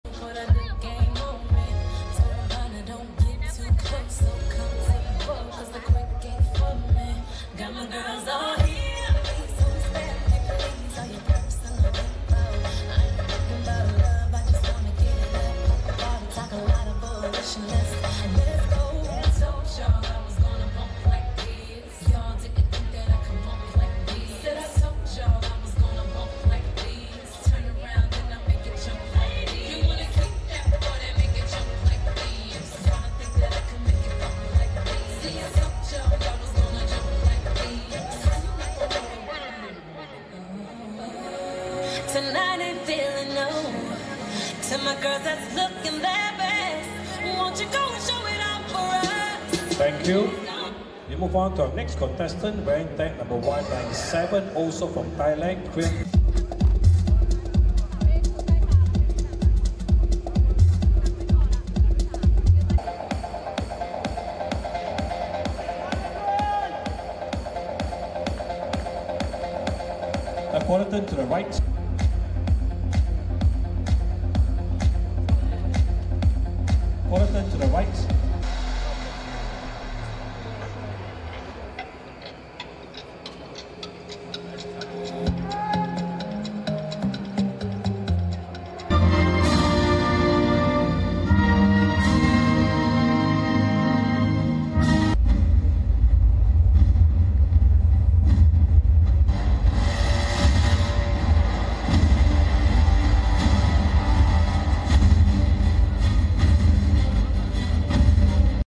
Men's Sport Physique @Kevin Champions
ได้ที่ 3 เหรียญทองแดง ประเภท Men's Sport Model up to 170 cm. 🥉🏆🇹🇭

เป็นรุ่นโบนัสครับ เพราะแข่งวันอาทิตย์หลังจากผมแข่งฟิตเนสวันเสาร์เสร็จแล้ว ซ้อมโพส 3 จุด ก่อนขึ้นแข่ง 30 นาที ได้น้องป๊อปมาช่วยสอนโพส ตกใจมากที่เข้ารอบ final 5 คน และตกใจเข้าไปอีกเมื่อแข่งรอบชิงจบ ประกาศผลการแข่งขัน ผมได้อันดับที่ 3 🥉🏆🇹🇭

ขอขอบคุณสมาคมกีฬาเพาะกายและฟิตเนสแห่งประเทศไทย ที่เห็นศักยภาพของผม และให้โอกาสผม จนได้ผลลัพธ์เกินความคาดหมาย 🥉🏆🇹🇭